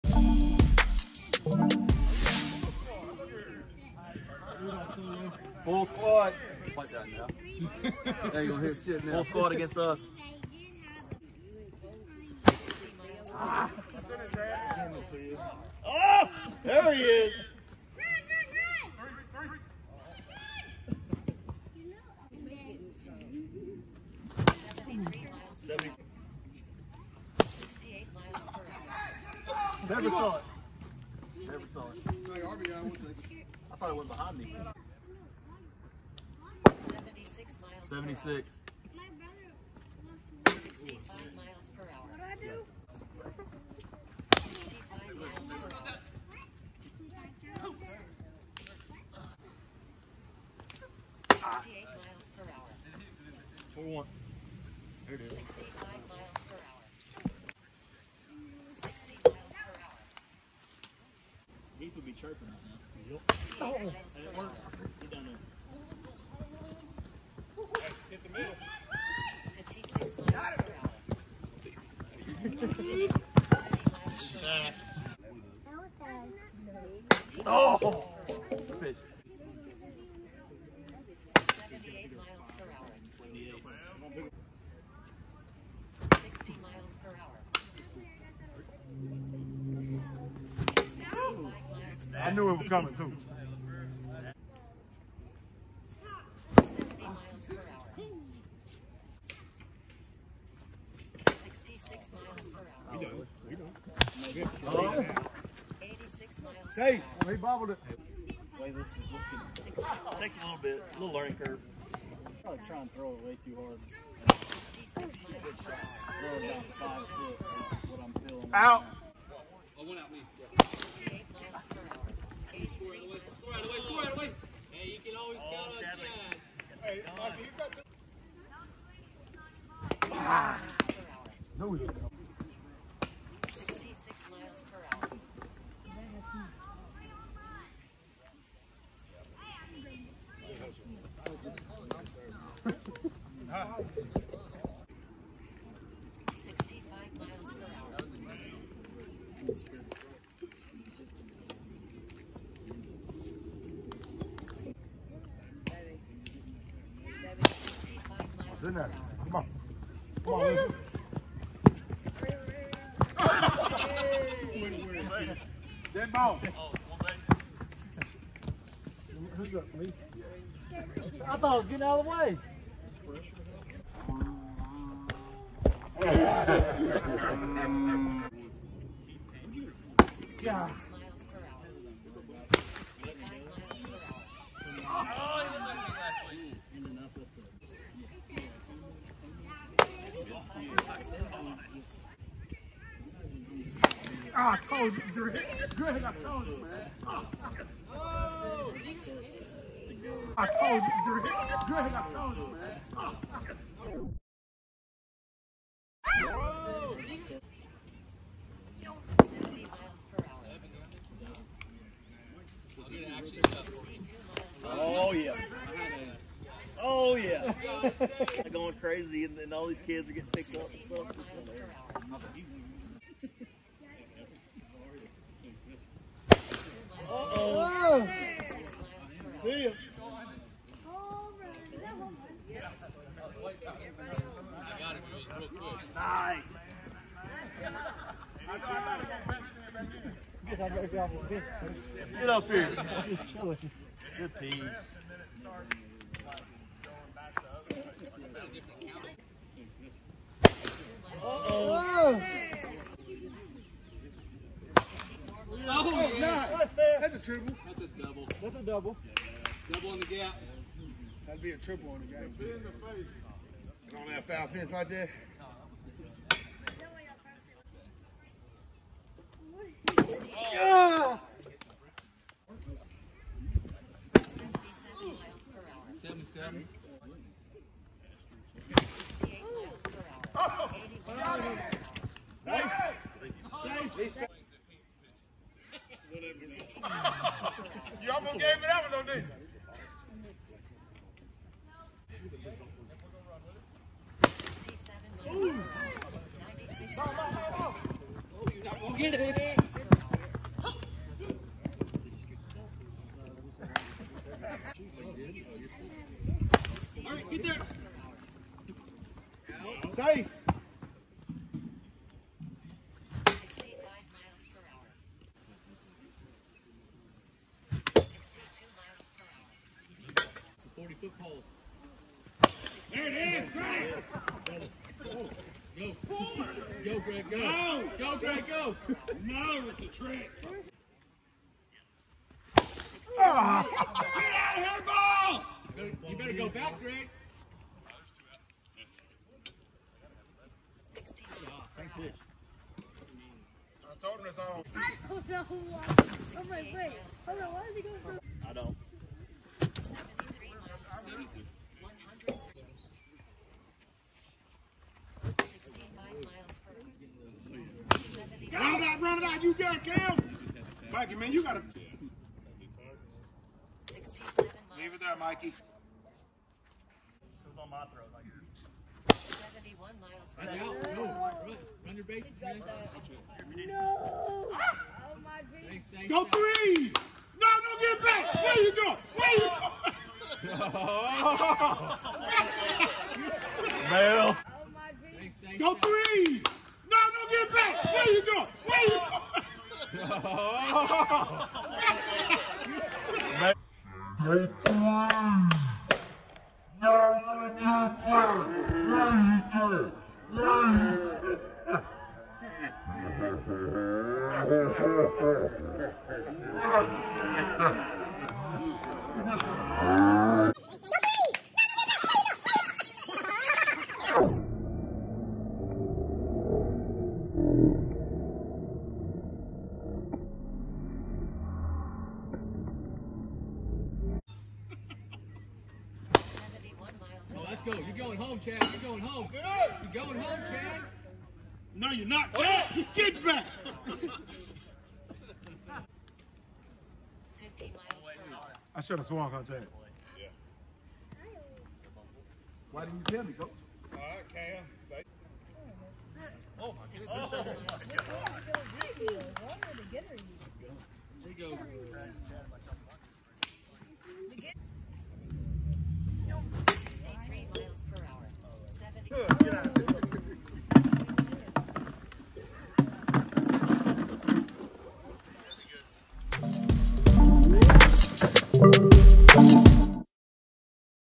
Diamond S Blitzball - Ruttin' Bucks vs Wardogs - 4-23-23
Bucks. Dogs. Rematch of the 2022 World Series Championship Game!